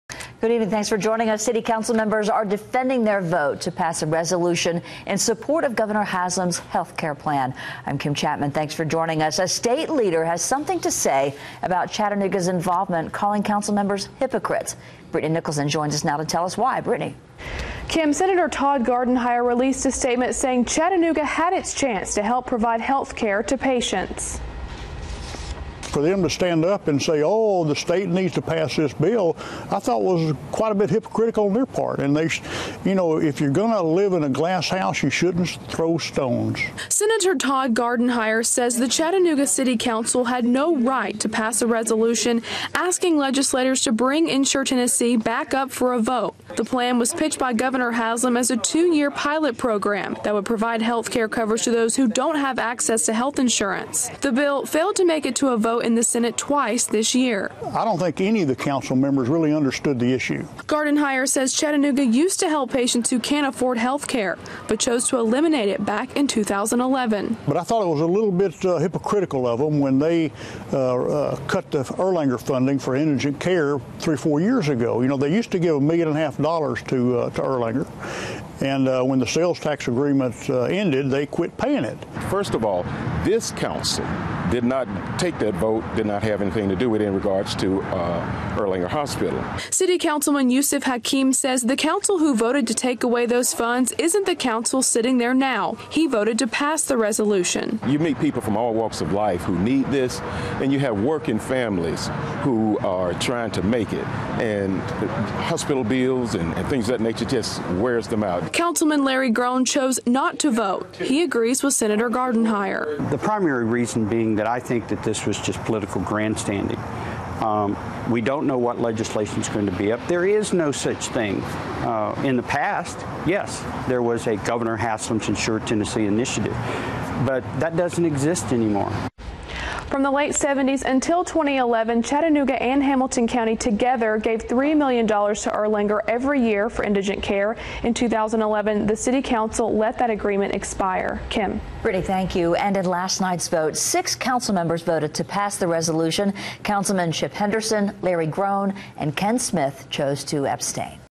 Senator Calls Chattanooga City Council Vote "Hypocritical"
Senator Todd Gardenhire released a statement, Wednesday, about a resolution the Chattanooga city council plans to send to state legislators.

Tuesday night, the city council passed a resolution in support of Governor Bill Haslam's Insure Tennessee healthcare plan.

The plan was pitched by Governor Haslam as a 2 year pilot program that would provide healthcare coverage to those who don't have access to health insurance.
The bill failed to make it to a vote in the Senate twice this year.

"For them to stand up and say 'Oh the state needs to pass this bill', I thought was quite a bit hypocritical on their part," Gardenhire said. "If you're going to live in a glass house, don't throw stones."

"I don't think any of the council members understood the issue," he went on to say.

Six council members voted to approve the resolution. Three members abstained from voting.

Gardenhire says Chattanooga used to help patients who can't afford healthcare but chose to eliminate it back in 2011.

"I thought it was a little bit hypocritical of them when they cut the Erlanger funding for indigent care 3 or 4 years ago, they used to give $1.5 million to Erlanger and when the sales tax agreement went through, they quit paying it," Gardenhire explained.

According to Gardenhire, from the late 1976 until 2011, Chattanooga and Hamilton county together gave $3 million to Erlanger every year for indigent care. In 2011, the city council let the agreement expire.

"First of all this council did not take that vote, did not have anything to do with it in regards to Erlanger hospital," City councilman Yusuf Hakeem said.

Hakeem says the council who voted to take away those funds isn't the council sitting there now. He voted to pass the resolution.

"You meet people from all walks of life who need this and you have working families who are trying to make it, and hospital bills and things like that just wears them out," Hakeem said.

Councilman Larry Grohn chose not to vote. He agrees with Senator Gardenhire.

"The primary reason being that I think this was just political grandstanding," Grohn said. "We don't know what legislation is going to be up, there's no such thing. In the past, yes, there was a Governor's Insure Tennessee plan, but that doesn't exist anymore."

Councilmen Chip Henderson, Larry Grohn, and Ken Smith chose to abstain from voting.

By: Brittany Nicholson